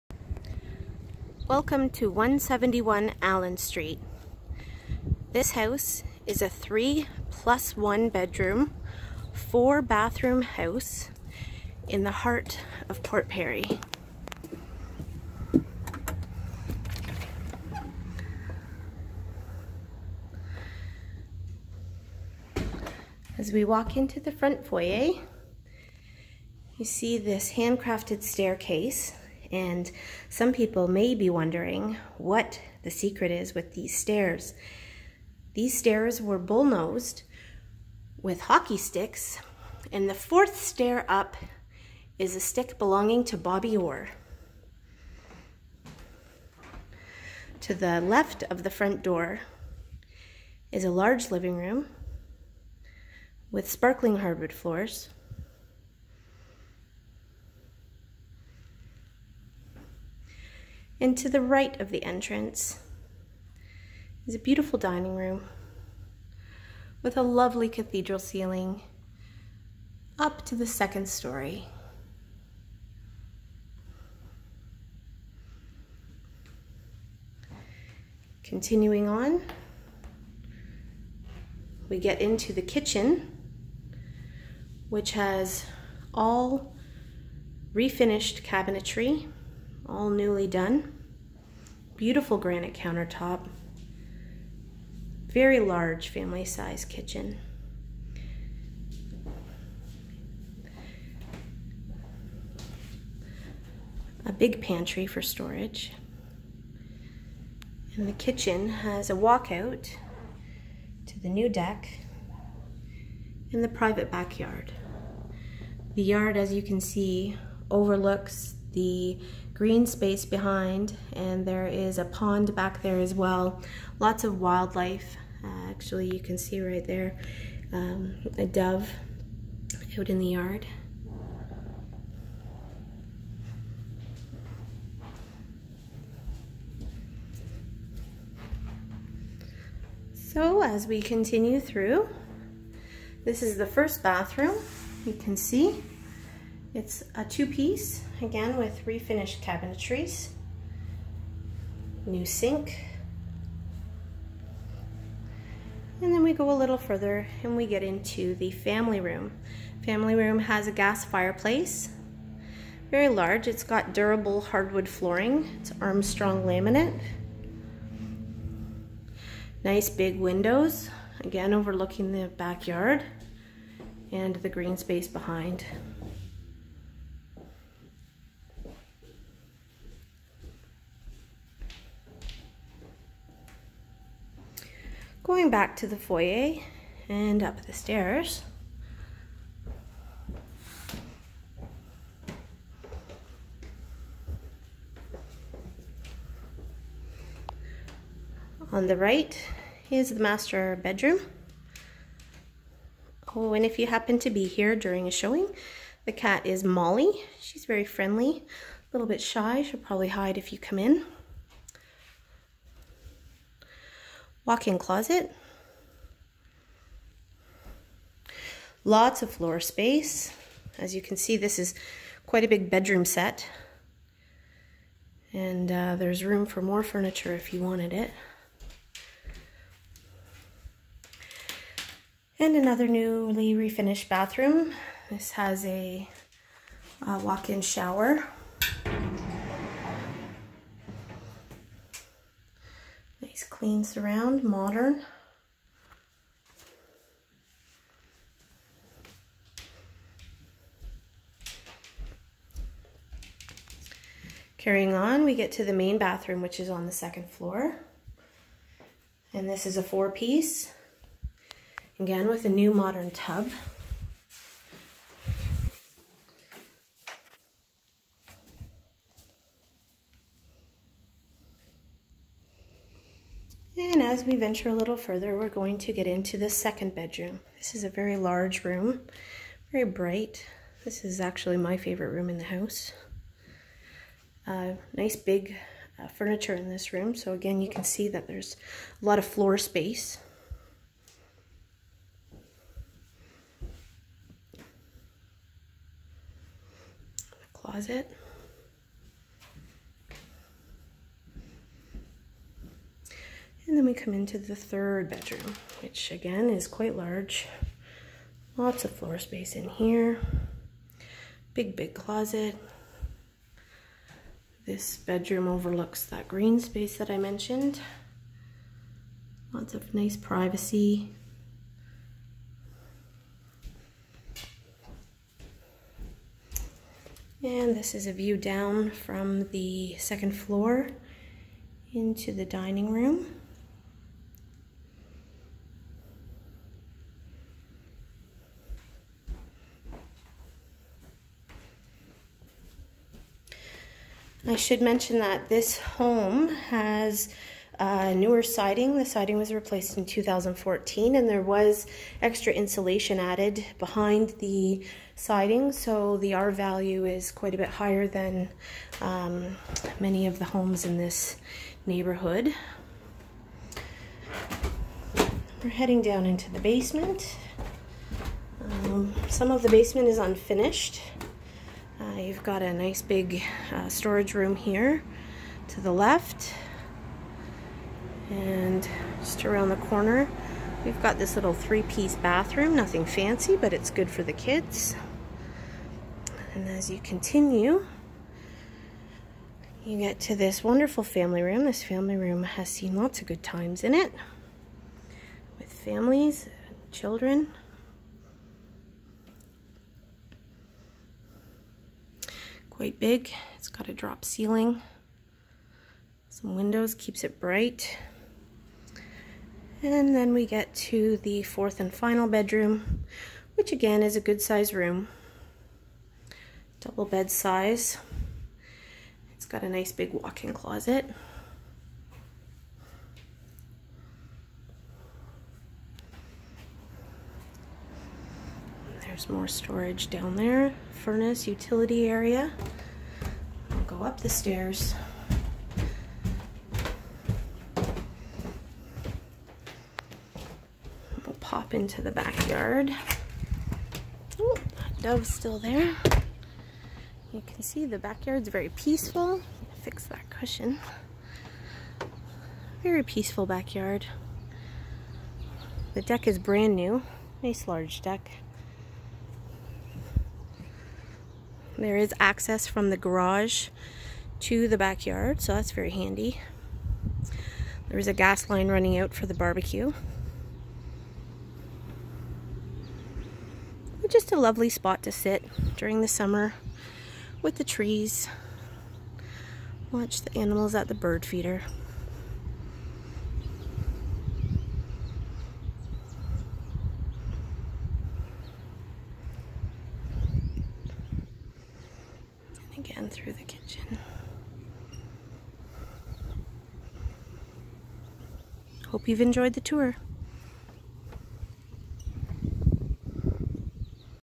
171 Allan Street, Port Perry
The house with the white picket fence! 171 Allan Street is a 3+1 Bedroom, 4 bathroom home for sale in the heart of Port Perry, Ontario - a quick walk to public and high schools, and historic down-town. Meticulously renovated, large family home backs onto peaceful greenspace.  Approximately 2000sq/ft.  This home is full of character.  Stunning new kitchen and bathrooms in 2019. Freshly painted throughout. Most windows are new. Siding was upgraded in 2014 with added insulation.

Too many upgrades to list:  Fully insulated garage (2019), Furnace (2014), Air Conditioning (2020), Owned Hot Water Tank, Shingles (2019), Interlock walkway (2020), Deck (2020), Most Windows, Garage Door